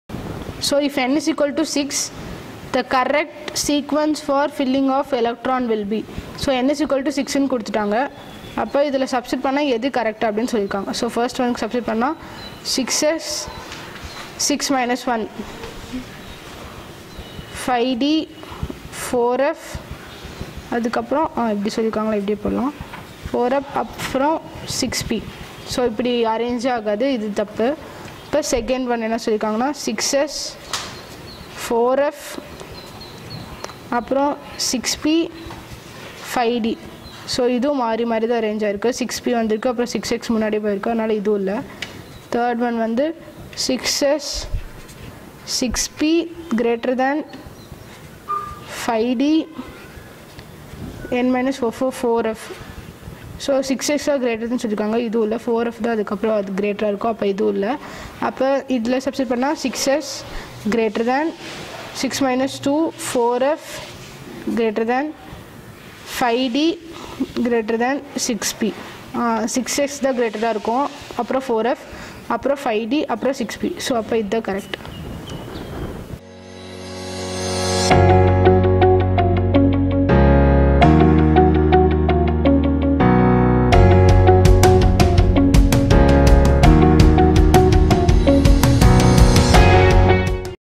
Atomic Structure neet mcq 14
If n=6, the correct sequence for filling of electrons will be

Atomic Structure - neet - mcq- questions and answers

Class 11 chemistry - neet - mcq - questions and answers

NCERT

Tamil